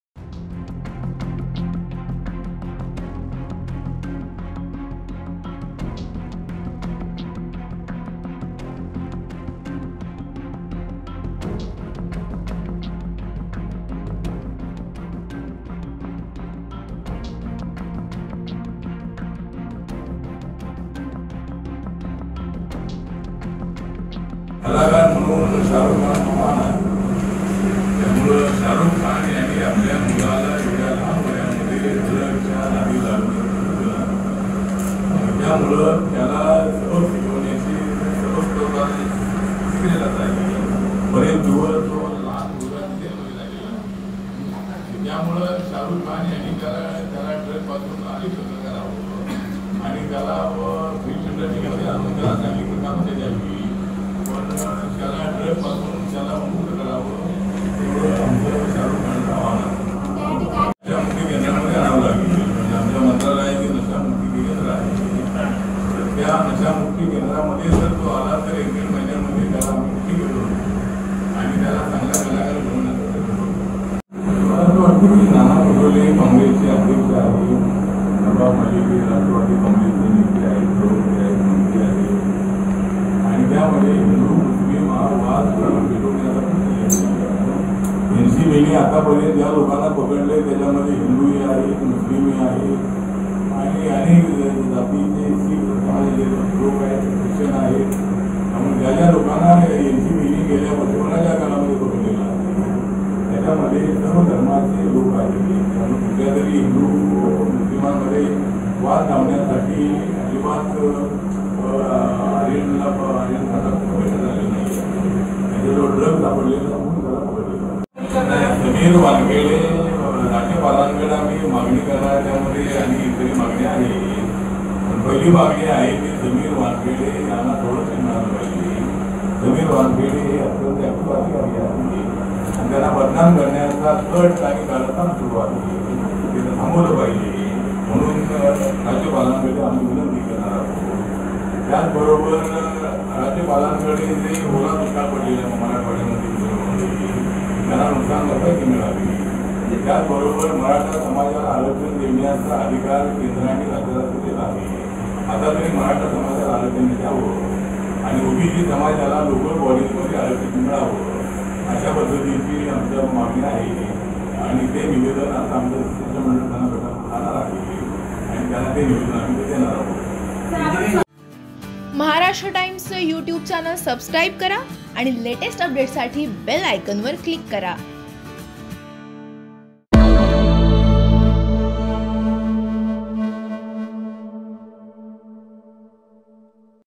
Ramdas Athawale : Sameer Wankhede यांच्या सुरक्षिततेसाठी राज्यपालांना भेटणार
सध्या आर्यन खान ड्रग्ज प्रकरण गाजत आहे. रोज नवनवीन आरोप प्रत्यारोप समोर येत आहेत. कलाकार म्हणून शाहरूख खान हे महान आहेत असं रामदास आठवले म्हणाले. रामदास आठवले यांनी आर्यन खान याने ड्रग्जपासून अलिप्त ठेवावं असं आवाहन केलंय. आर्यनला नशामुक्ती केंद्रात पाठवावं असा सल्ला आठवलेंनी दिला आहे.